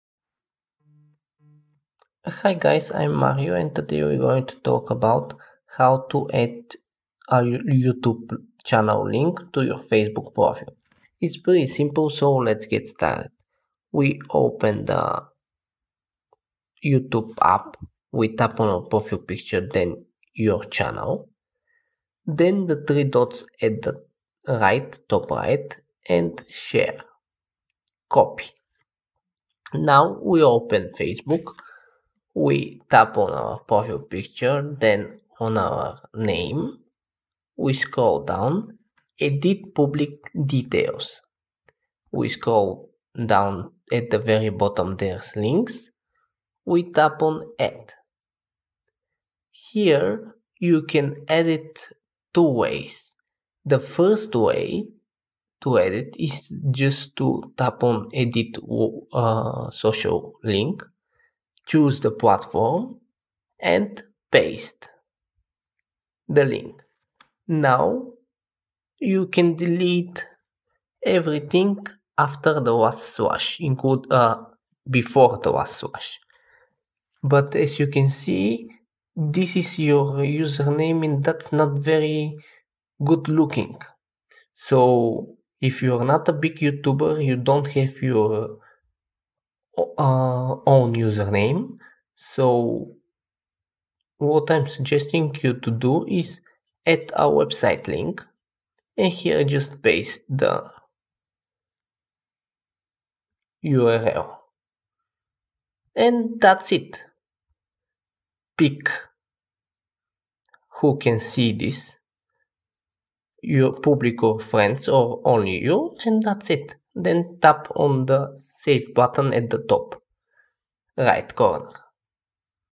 Add Your YouTube Channel To Facebook Profile
It's fast and easy to add a YouTube channel to your Facebook profile.